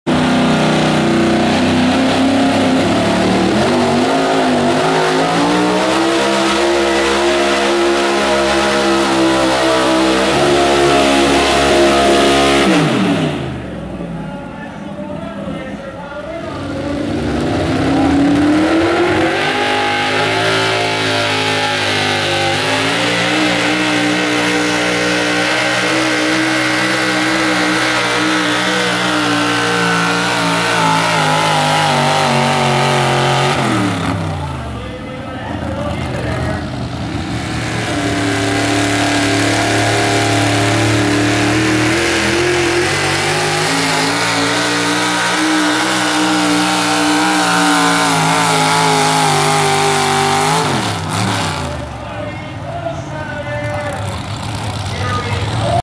FPP, Lawrence Cty, Pro Street GAS, Pulloff, 8/18/11
1   Ron Cutrell 89 Chevy Parts and Pieces FP/267.77
2   Jeff Mitchell 77 Ford Tragic Magic FP/264.35
3   Mark Petros 75 Dodge Yellow FP/254.96
4   Matt Moser 90 Ford Marwood Exp.  FP/BROKE
5   Rob Forell 76 Ford Legal Addiction 297.88
6   Craig Bell 76 Ford Black 297.36
7   Dennis Nichelson 71 Chevy Wild Thing 291.7
8   George Nichelson 57 GMC Green 287.09
9   Ronnie Unger 85 Chevy Unger Taker 245.38